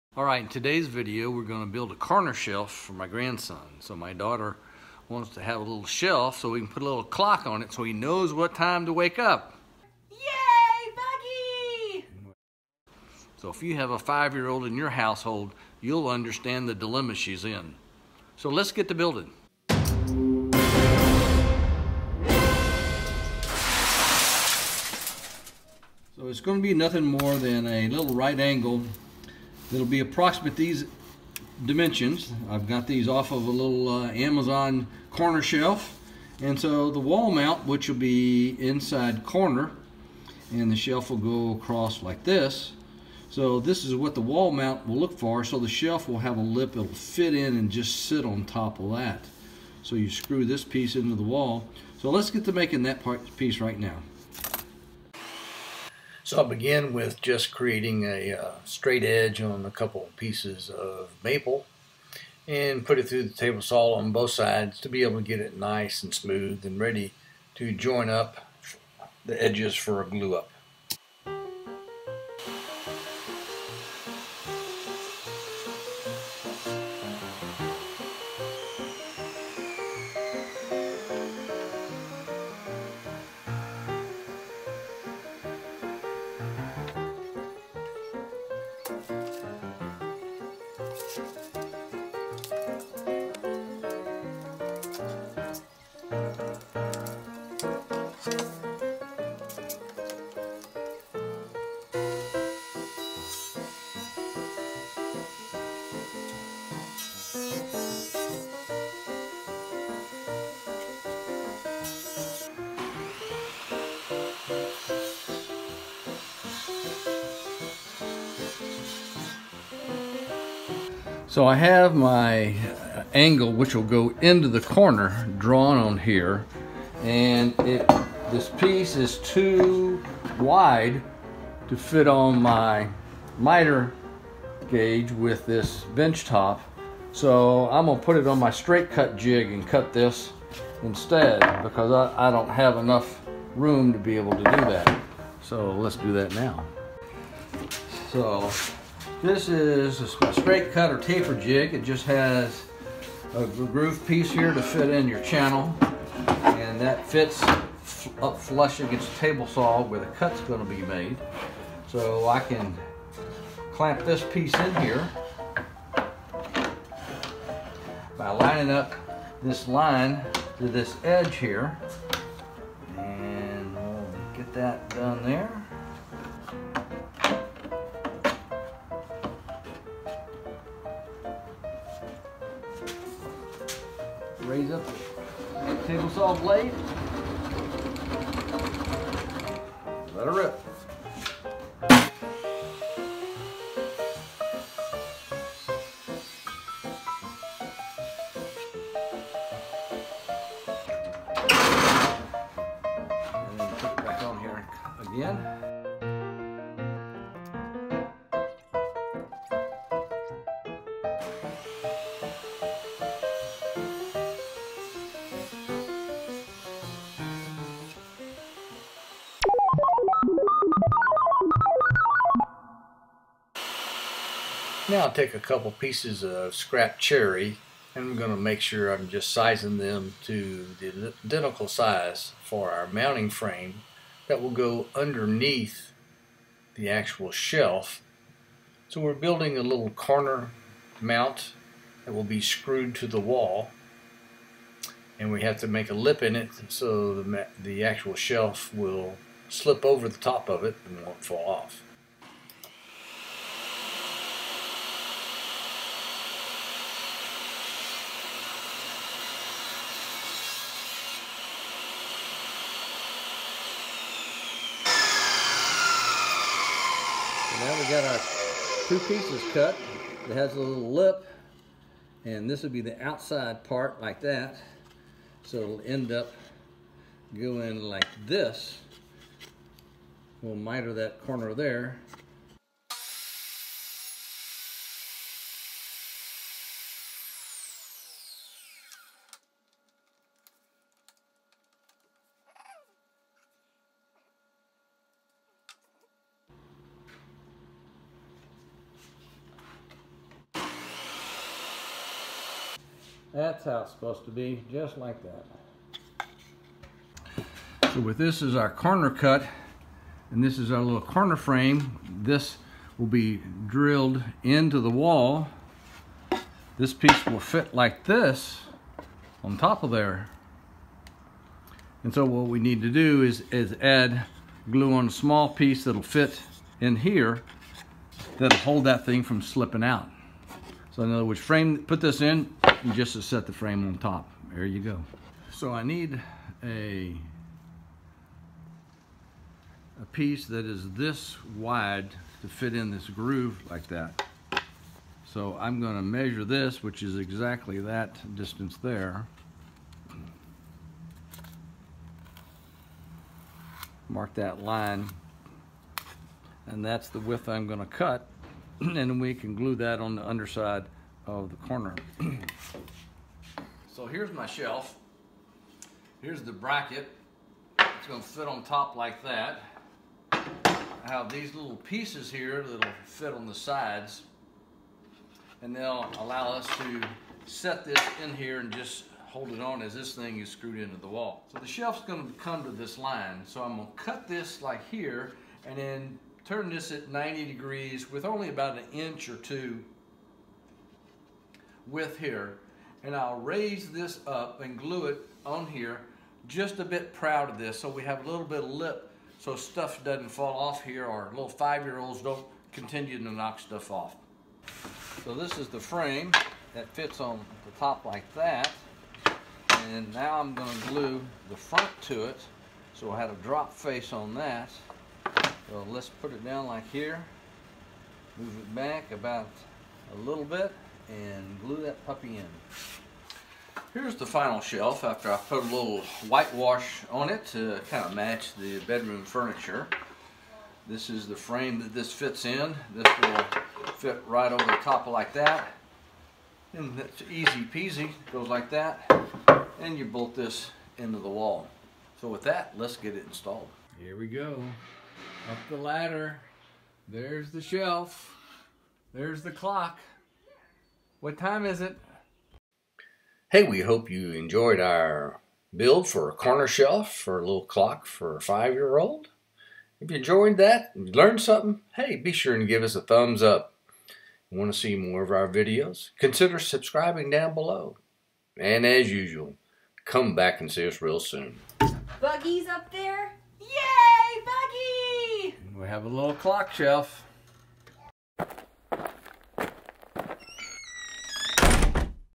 How to Build a Corner Shelf
Sometimes you just have to make it custom, as was the case with this shelf for my grandson's room.  When a five year olds wants to get up too early for Mom & Dad, you have to teach them to stay in bed until the alarm clock tells them it is time to get up.  So where do you put this clock?  Just high enough and in the corner, out of the way.  But, we all know that nothing is out of reach for a five year old.  None the less, I built this shelf for the clock to fit in the corner.

To see my Tool list on Amazon,

Dozuki Japanese Saw
Glubot, glue bottle
Dowel Centers
Brad Point Drill Bits, set of 6